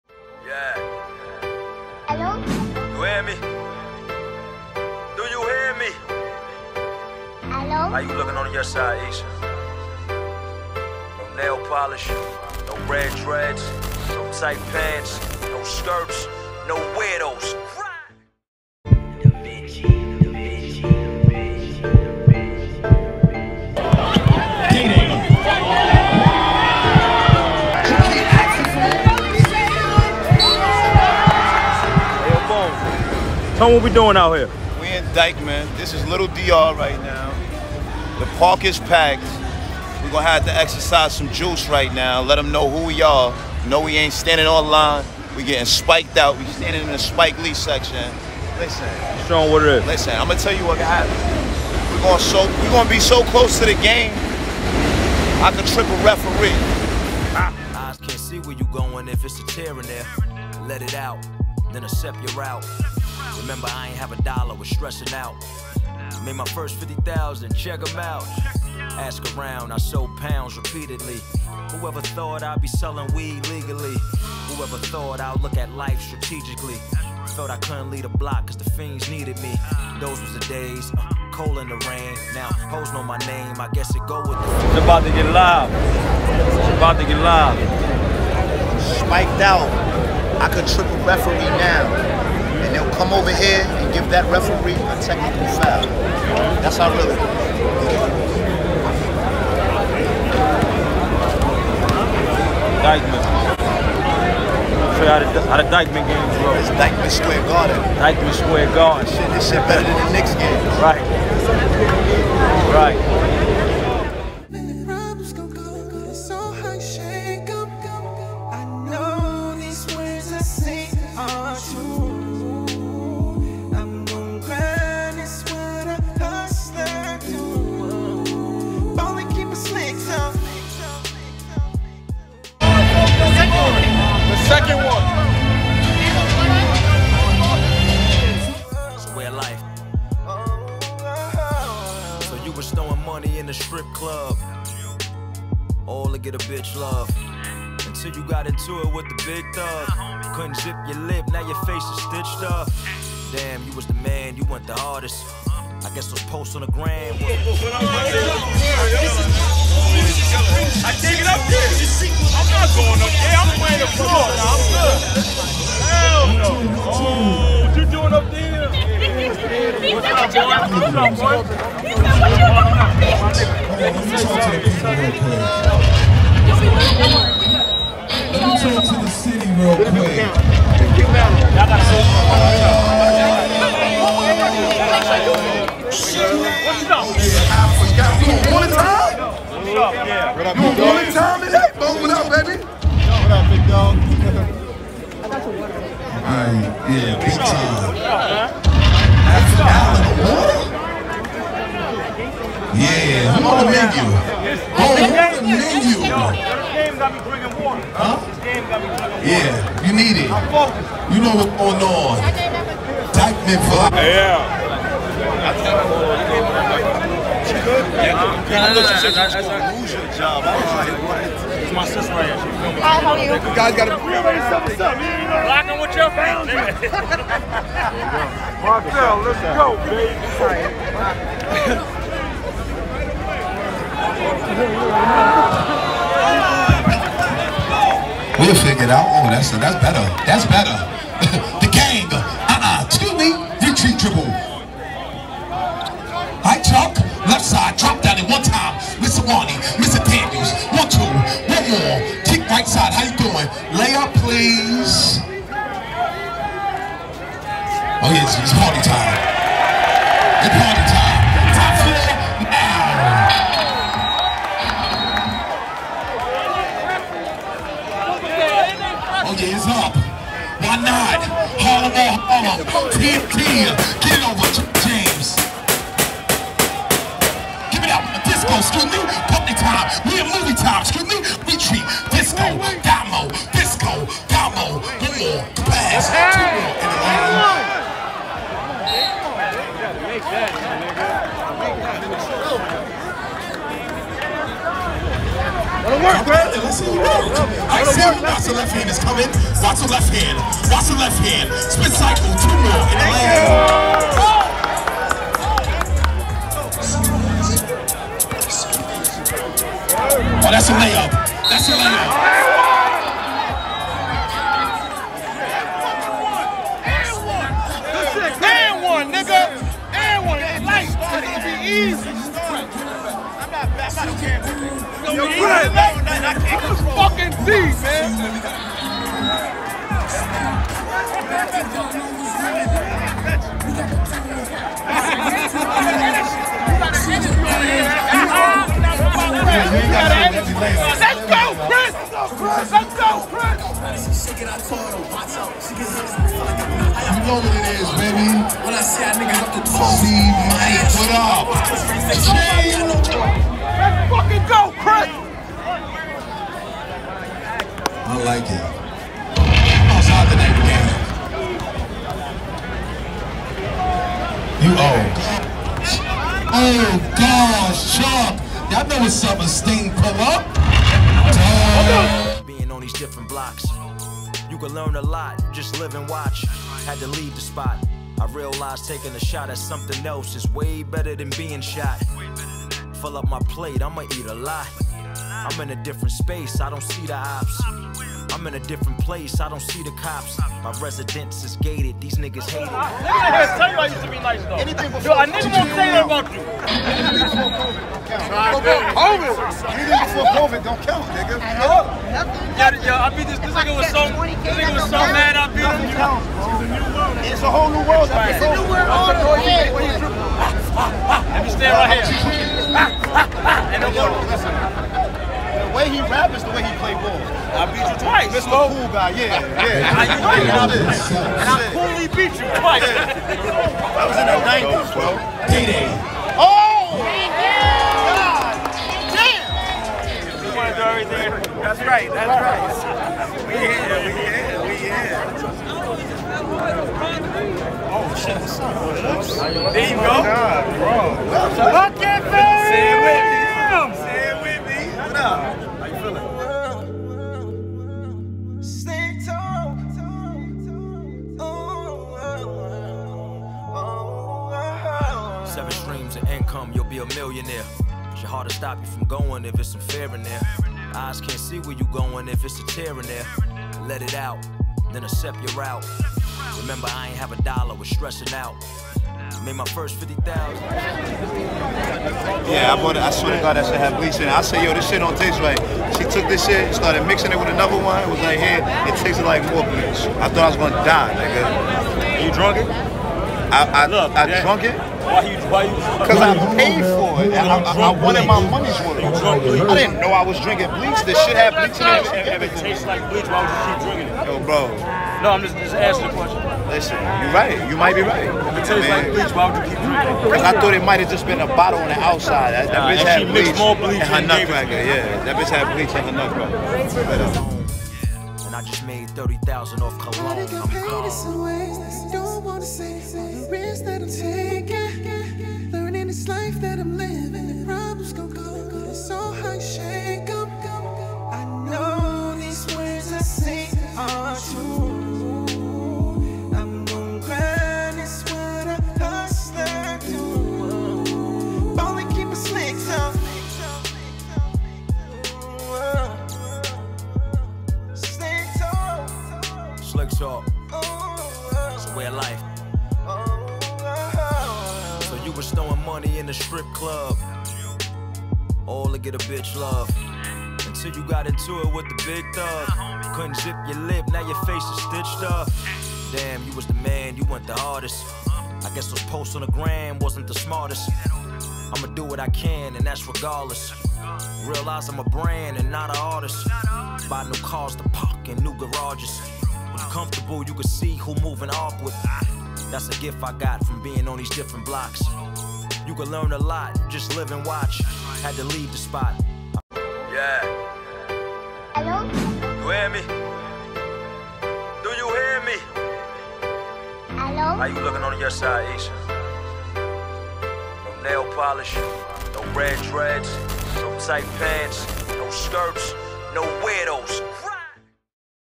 DYKMEN SQ GARDEN " LIFE IS GOODZ " Episode 3
Championship Game At Dykmen... The biggest basketball Tournament in NEW YORK￼